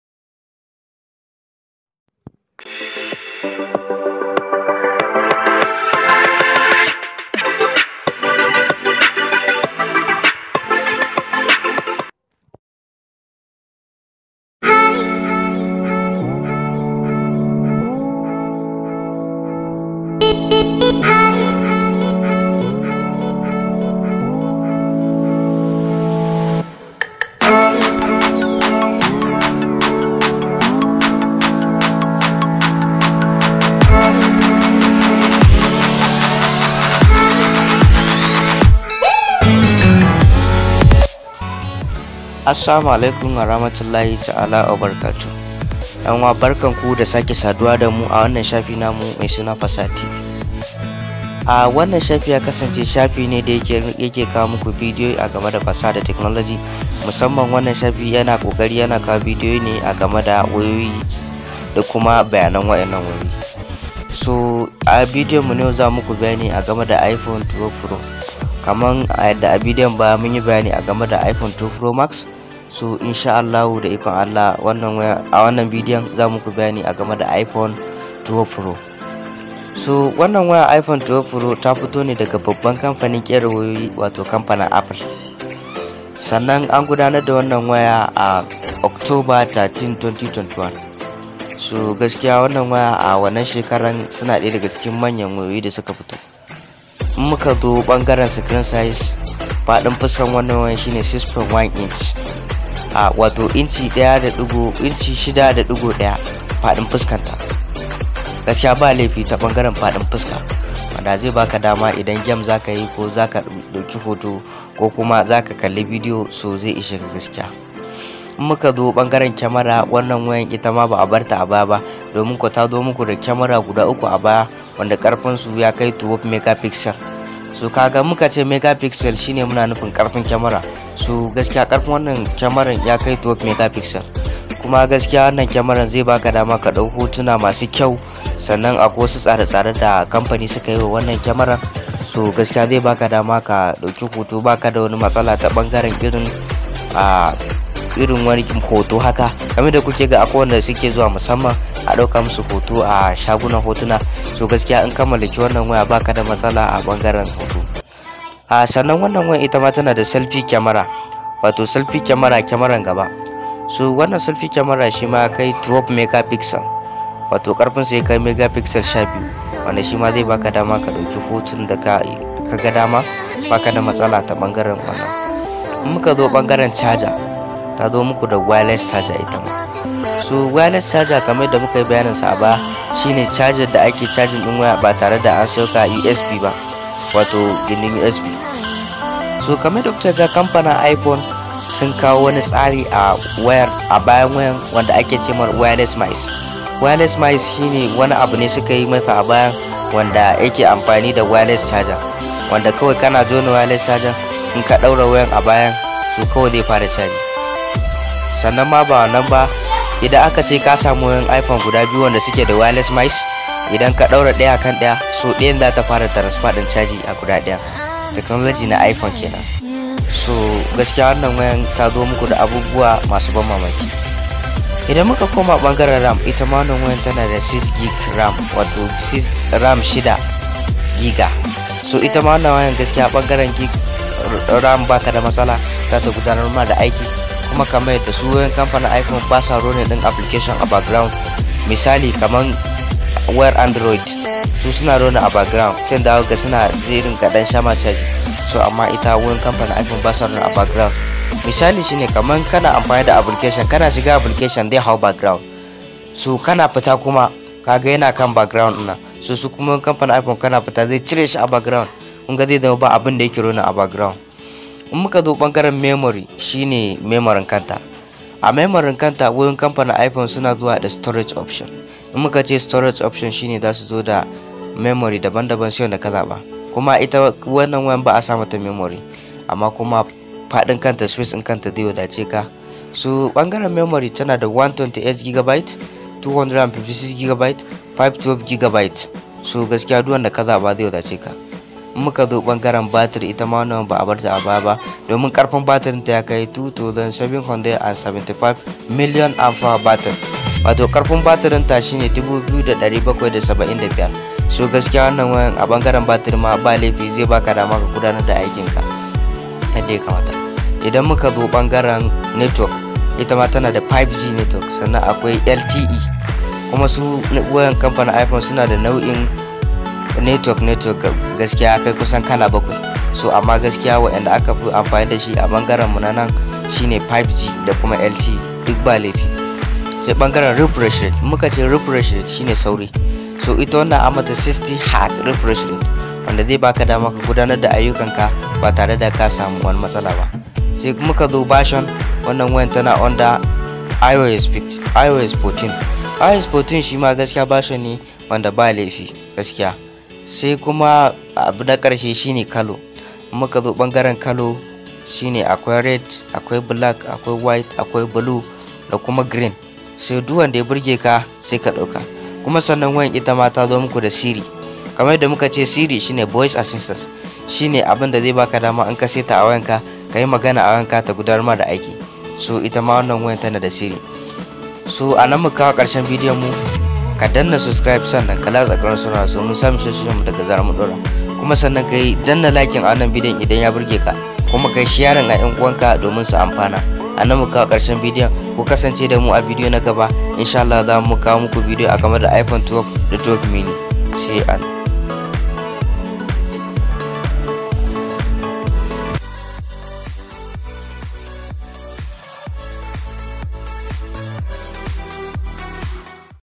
IPHONE 12 PRO 540p
Assal barka da zuwa fasaha tv wannan video zai muku bayani akan sabuwar wayar da ta shiga insha Allahu iPhone 12 pro